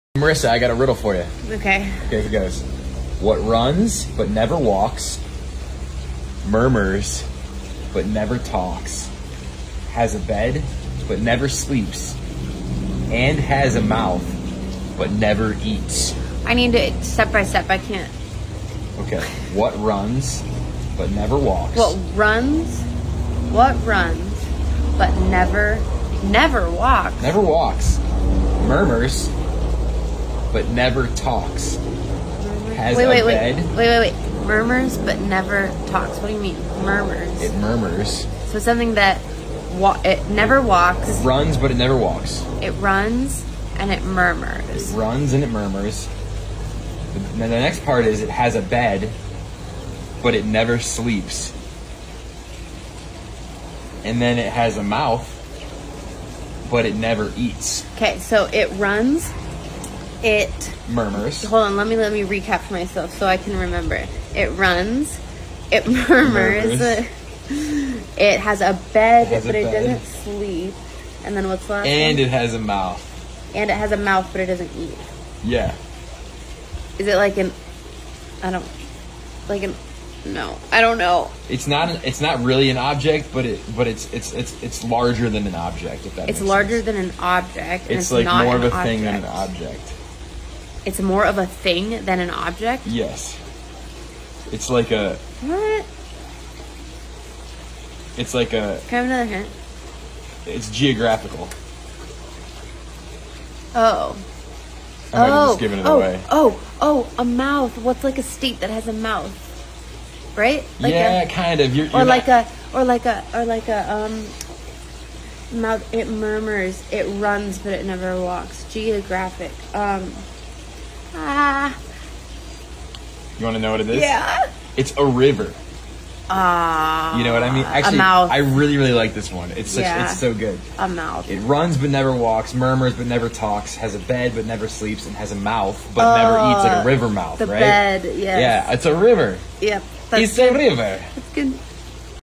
My girlfriend could not guess this riddle
She was so stumped even though she tried her hardest